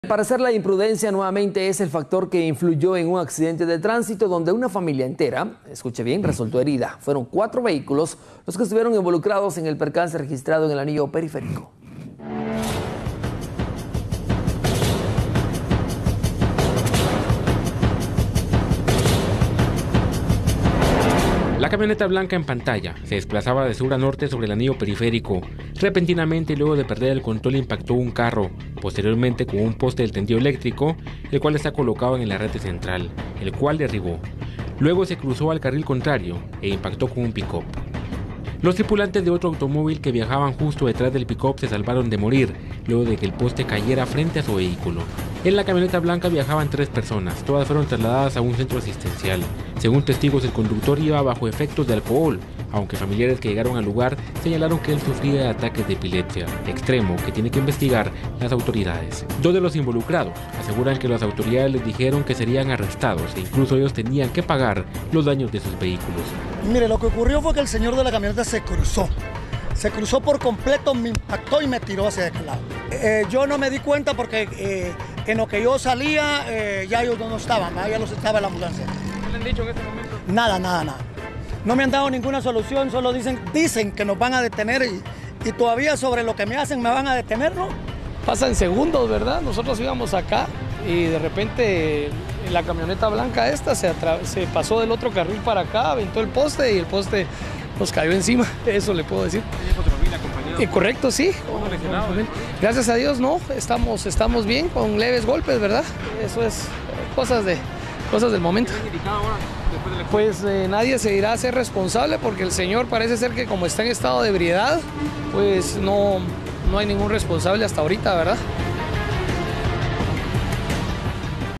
Triple colisión en anillo periférico
Una camioneta blanca se desplazaba de sur a norte por el en anillo periférico, luego de perder el control impactó a un carro, posteriormente con un poste del tendido eléctrico y para finalizar en el carril contrario impactando con un pick up.

En la camioneta blanca viajaban 3 personas